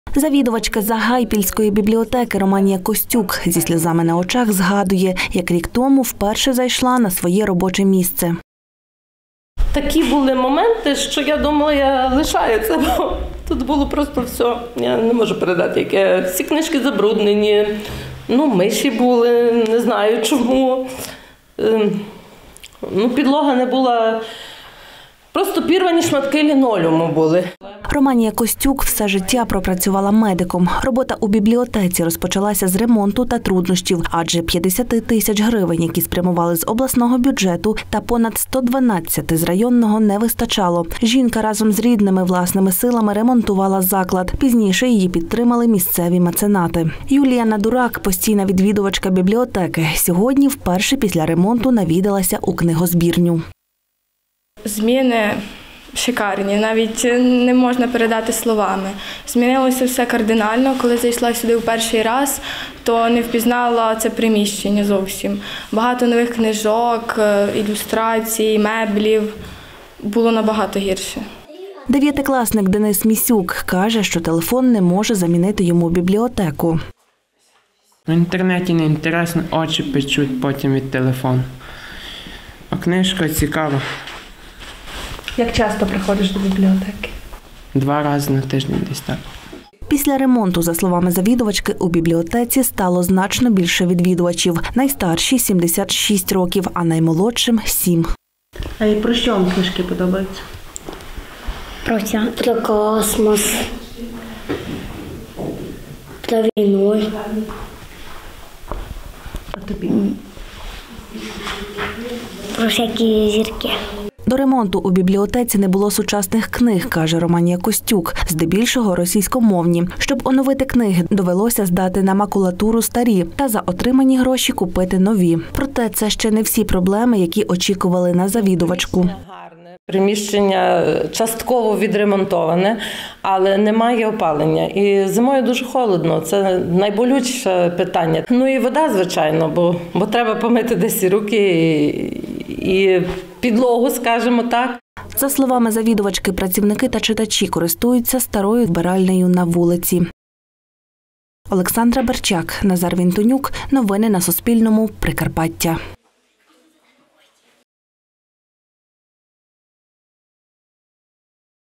Прикарпатка разом з родичами відремонтувала сільську бібліотеку
Зробили ремонт, закупили нові меблі та сучасні книги - у Загайполі Коломийського району відкрили оновлену бібліотеку. Керівниця закладу Романія Костюк зініціювала ремонт та разом з родичами власними силами ремонтувала приміщення. Адже грошей, які виділили з бюджету, не вистачало.